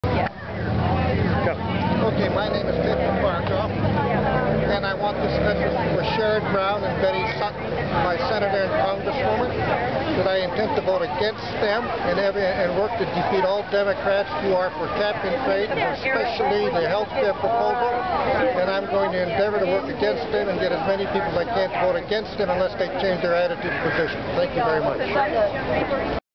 Ted C Cuyahoga Falls Ohio - Betty Sutton constituent
While at the Akron Tea Party: Ted C of Cuyahoga Falls Ohio tells Congresswoman Betty Sutton to vote NO on the Health Care Bill!